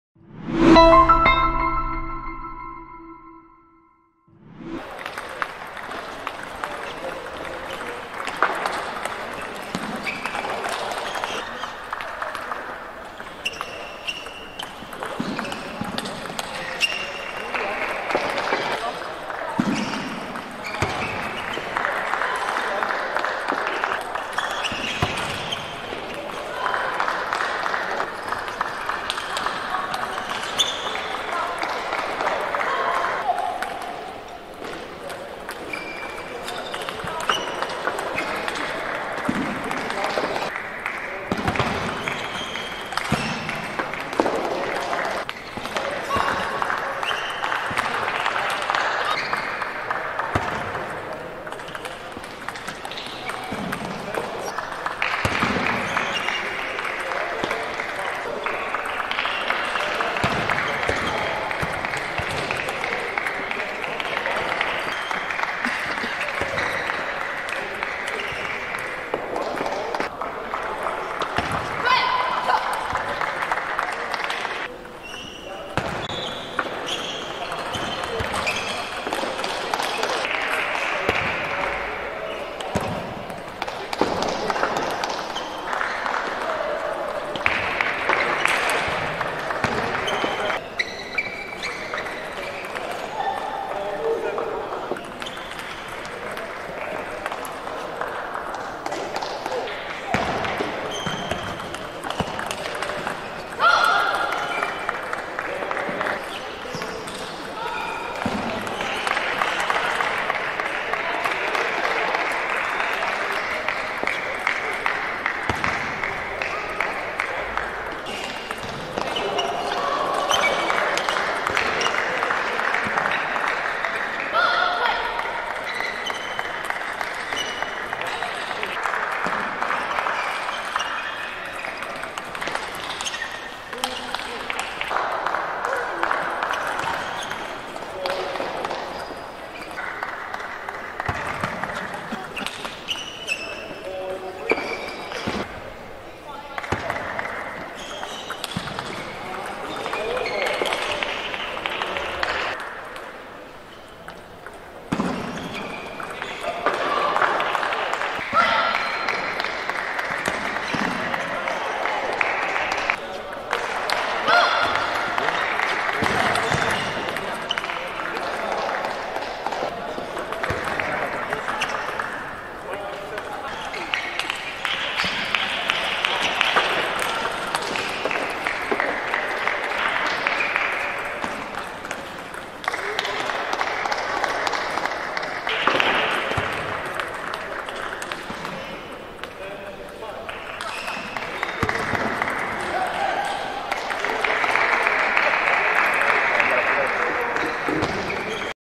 Judith Cobas vs Fuwa Yumoto | 2019 ITTF Spanish Open Highlights (Group)
Review all the highlights from the Judith Cobas vs Fuwa Yumoto at the 2020 ITTF World Tour Challenge, Spanish Open